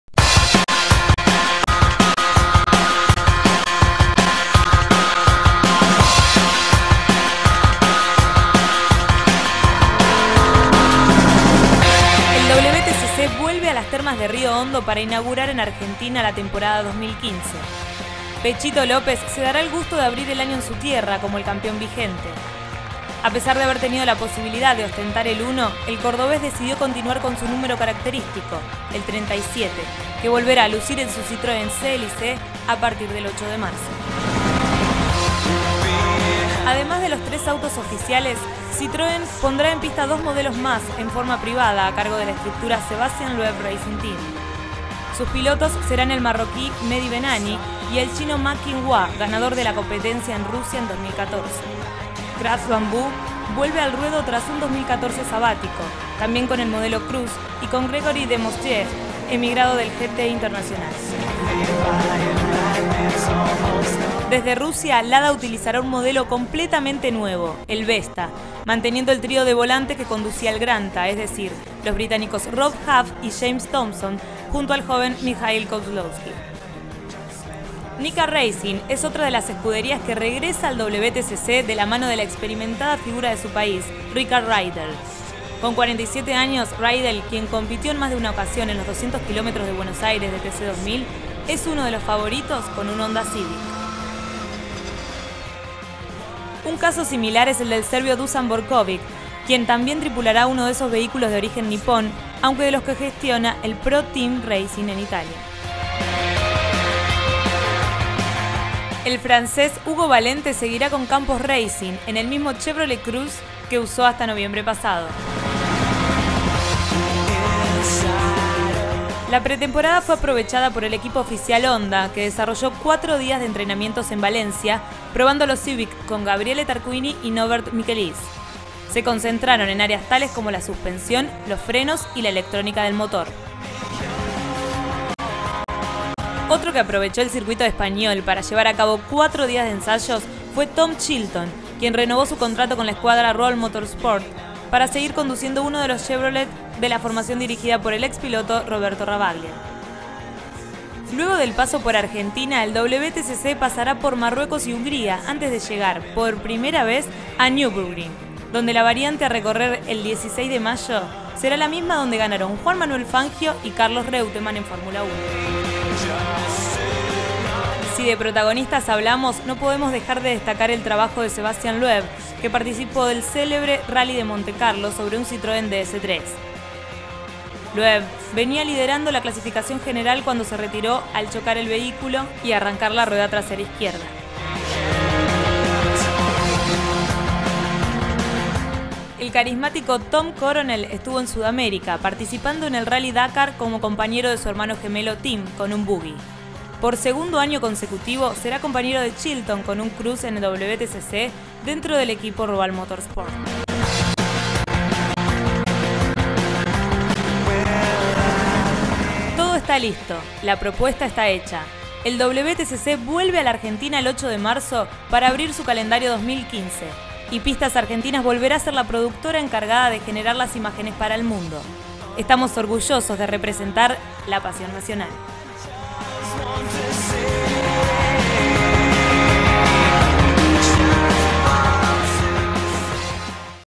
Vuelve el WTCC a Argentina
El WTCC abrirá su temporada 2015 en Las Termas de Río Hondo. El 6, 7 y 8 de marzo compartirá escenario con el Turismo Nacional.